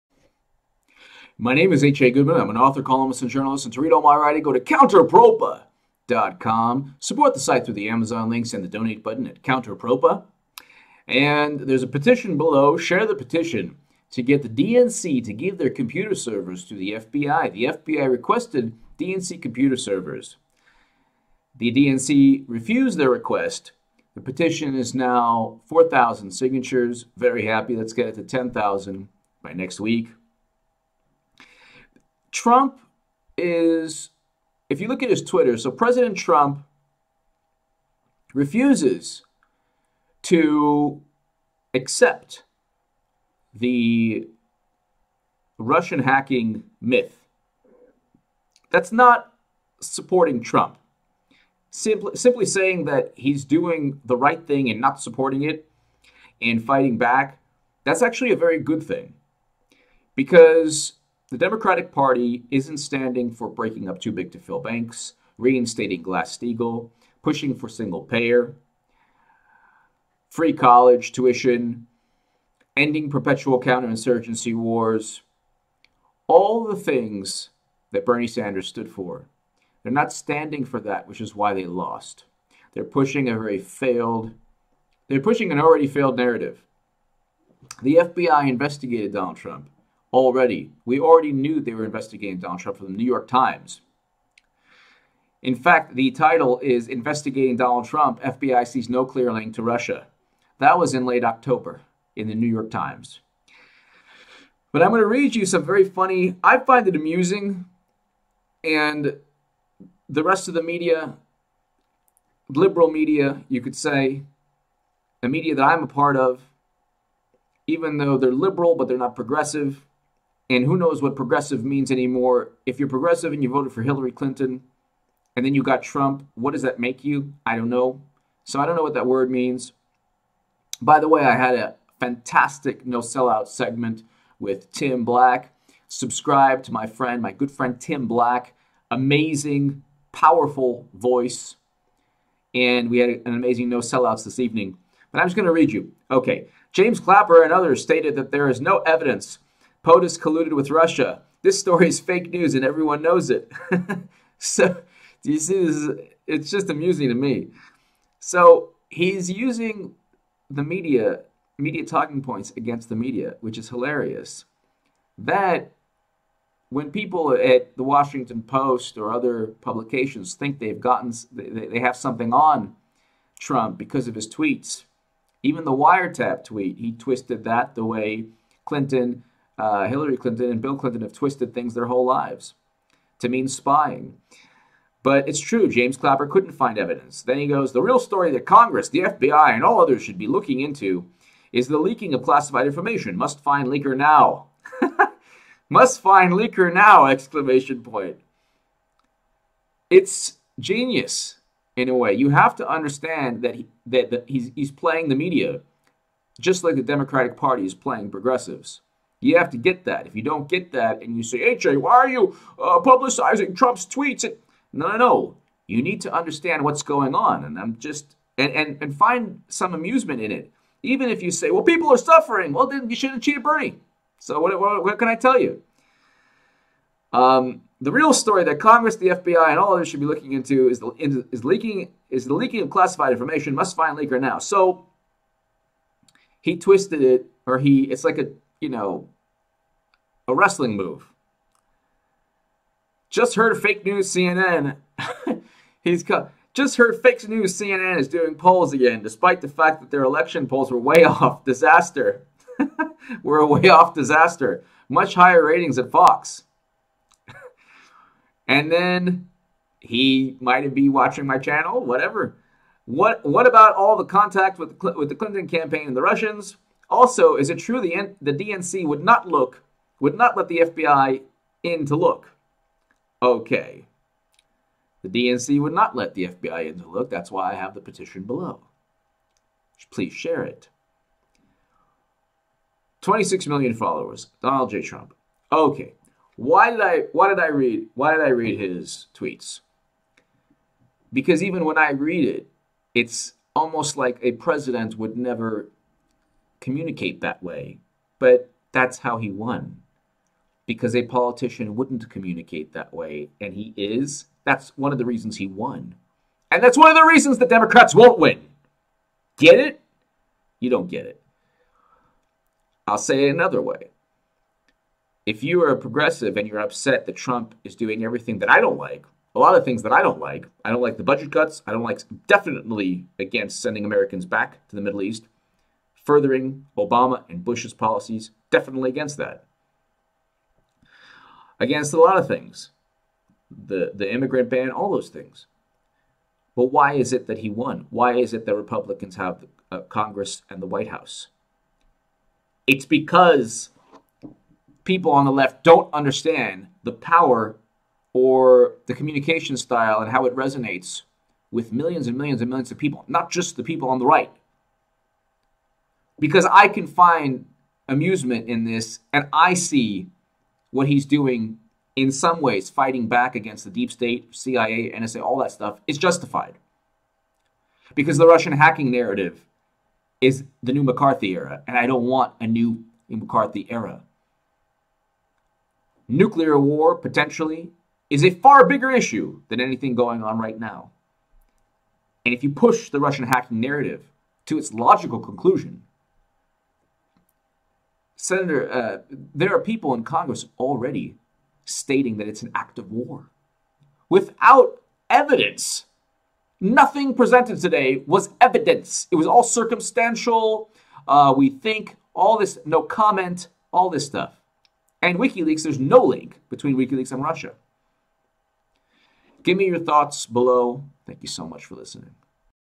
TRUMP HITS BACK AT FBI, ''NSA RUSSIA'' HEARING VIA TWITTER Democrats Will Lose Again
Trump is an anomaly.  We've never had a president that didn't go along with the status quo.  Republicans, democrats, independents et al have no idea what to make of him.  He is the first president I can remember (I'm 59) that is actually doing what he said he would do.  I love this guy.  Anyone who makes everyone mad, from both sides of the aisle, can't be bad.  I've already written my congressmen, all republicans in my state, and told them if they don't get on board the Trump train, I'll mount an effort to defeat them at their next election.  I'm excited about politics for the first time since the mid 90's.

Thanks for hanging tough against the opponents of the American citizens H.A.  Even though you may not think so, we're on the same side.  We may have a few differences, but we both believe in the freedom of America and defeating the elitists in Washington.

Also edited:  Please sign H.A.'s petition to get the DNC to turn over the server data to the FBI!  It's important!!